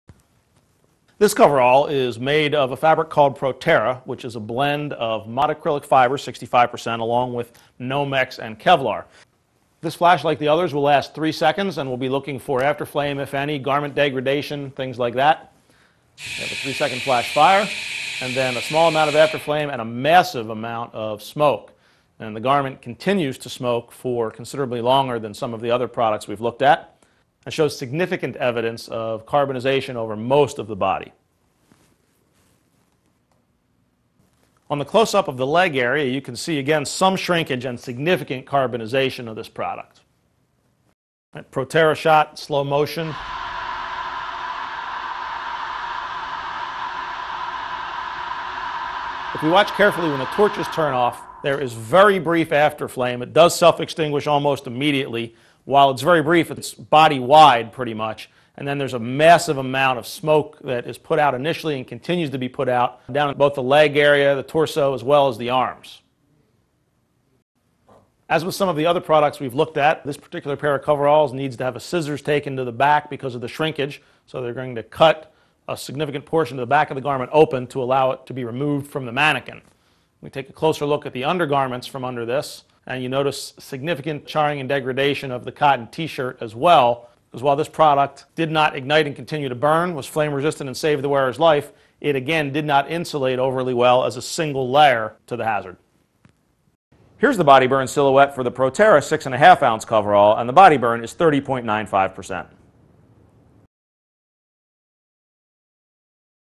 Protera® 6.5 oz. (220 g/m2)
Three-second flash fire test on manikin wearing Protera® coverall, showing resulting body and fabric burn.